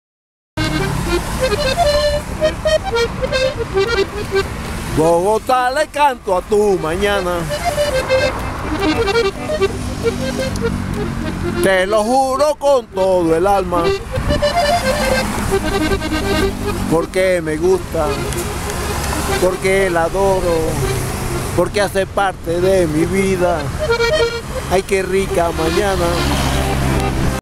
Le canto a Bogotá con el alma
Mi pasión es la de cantarle a Bogotá con todo el corazón.